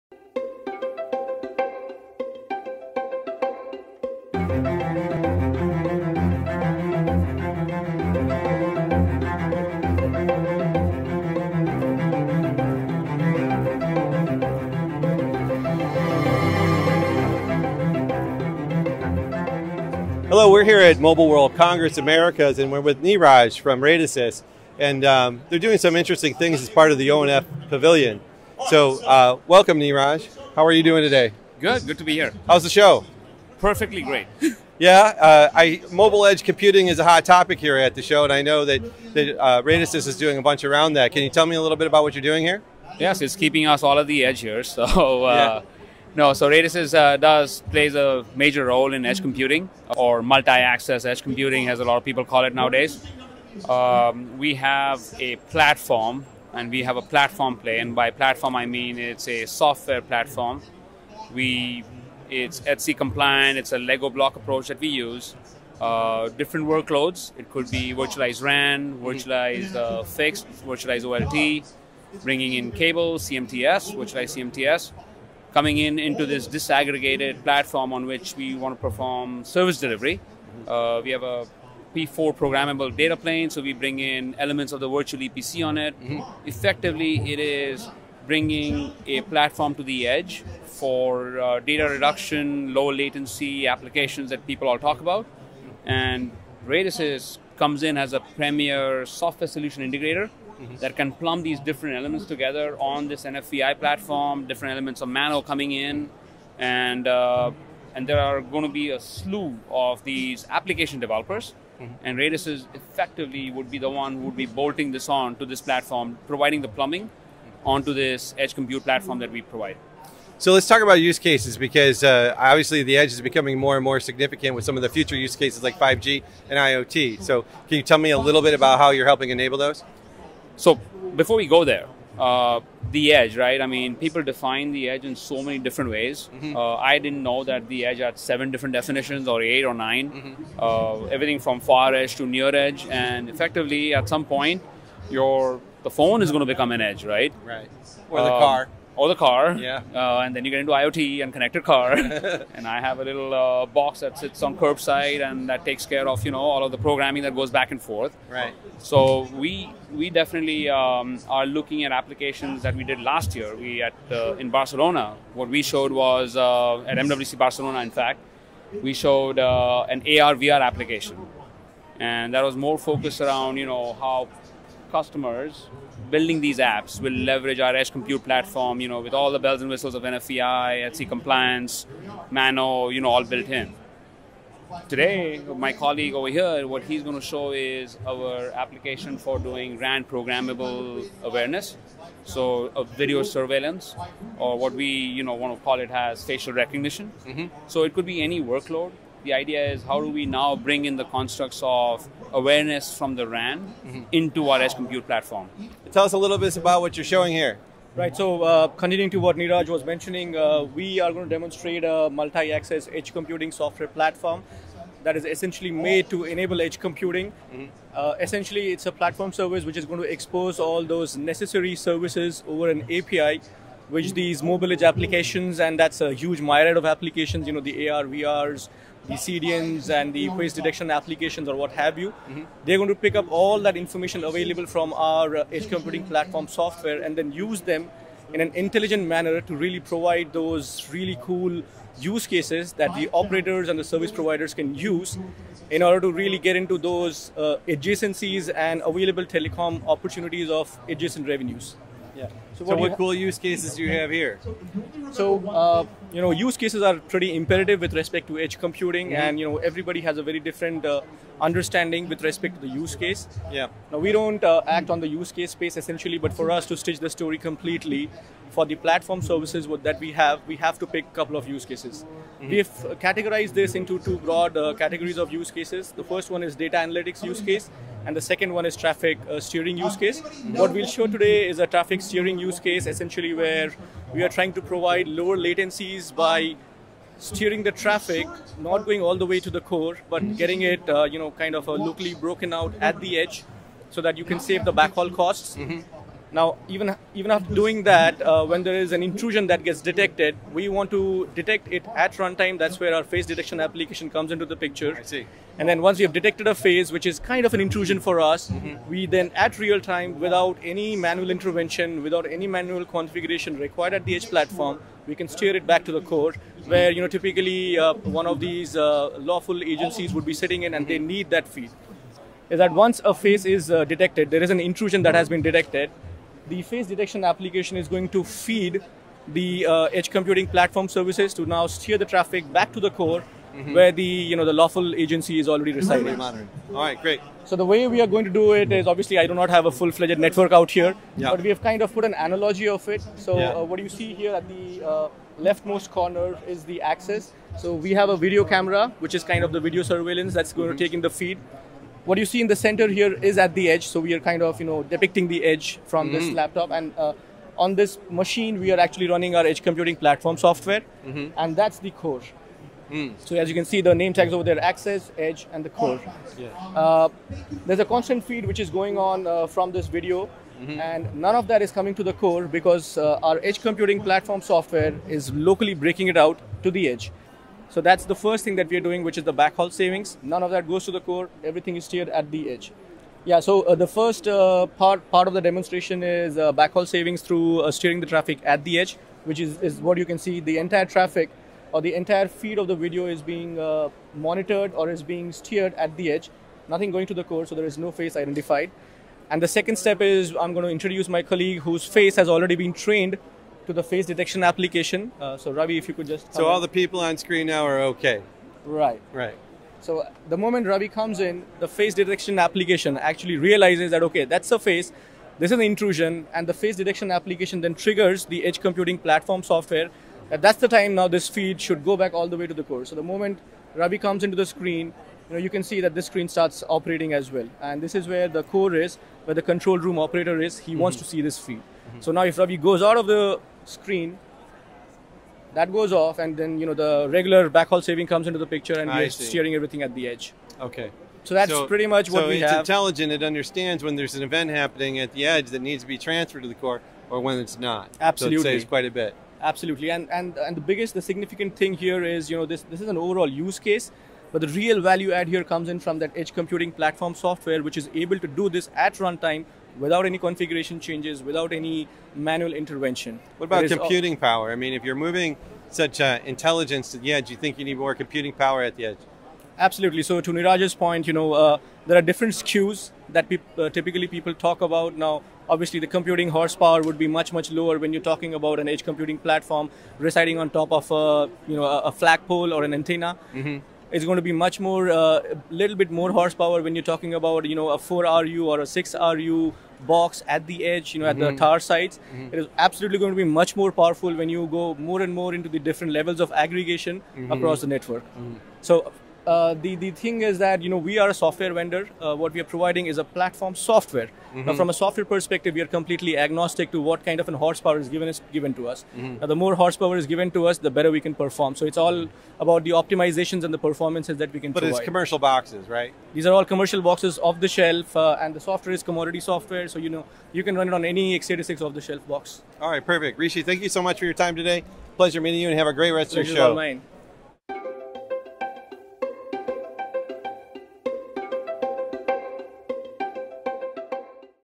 Radisys and Pipeline- Educational Demo at MWCA2018
This is a 9 minute long educational demonstration on MEC